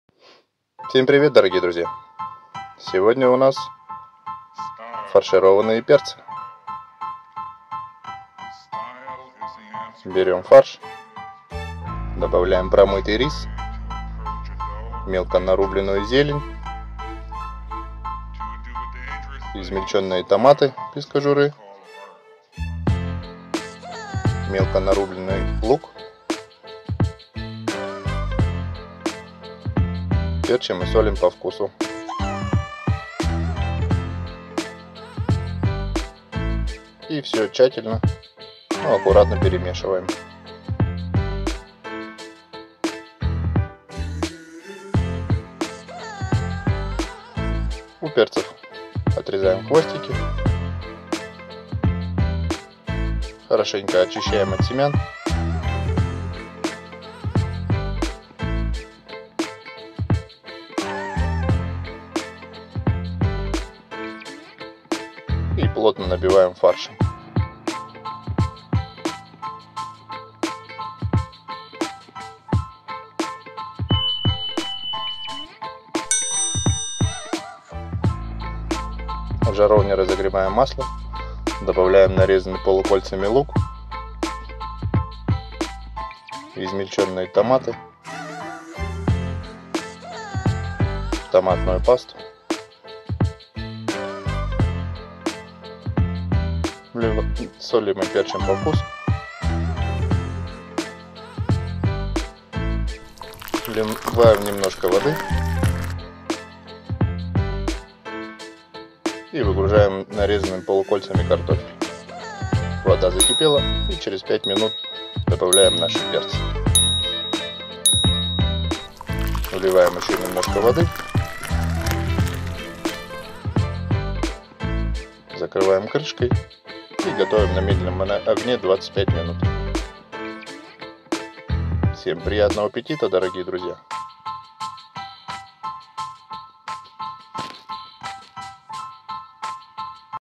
Фаршированные перцы
Всем привет🖐🏻

Сегодня готовим ФАРШИРОВАННЫЕ ПЕРЦЫ😋

НАМ ПОНАДОБИТСЯ:
-перцы 6 шт.
-фарш 450 гр.
-укроп пучок
-томат. паста 60 гр.